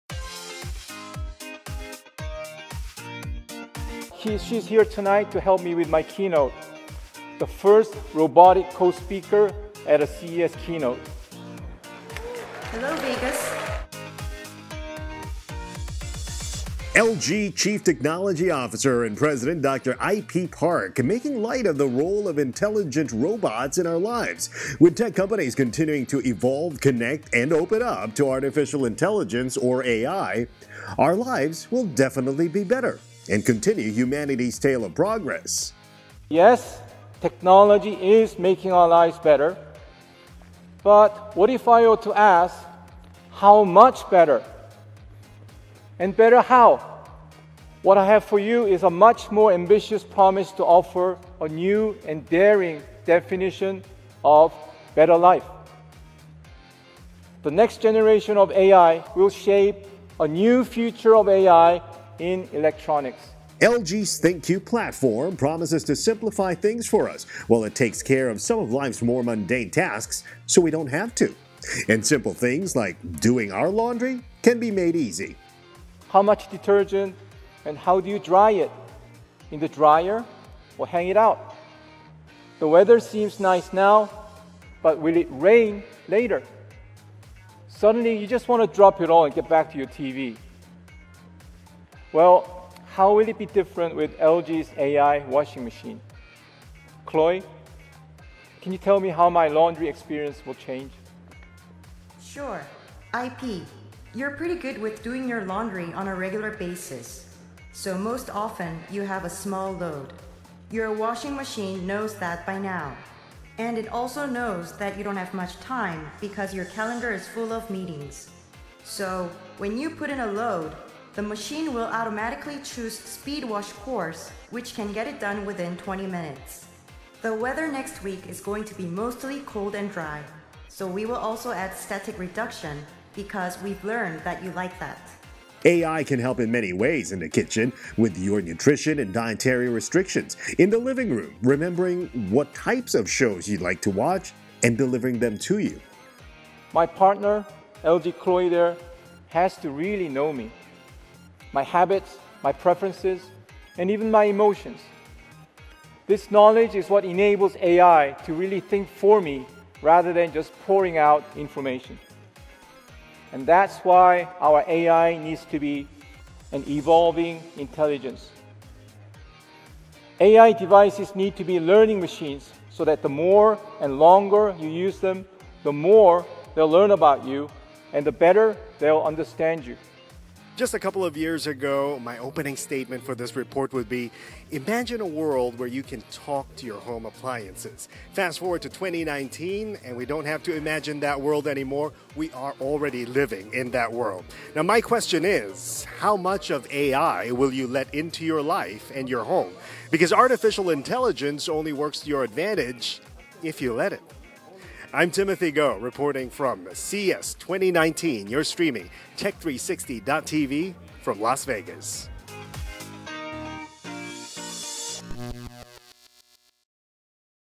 LG’s CES 2019 keynote: Using A.I. to better lives
In his keynote speech at the Consumer Electronics Show in Las Vegas, LG’s President and CTO IP Park shares his vision for artificial intelligence and how he believes it will simplify our lives.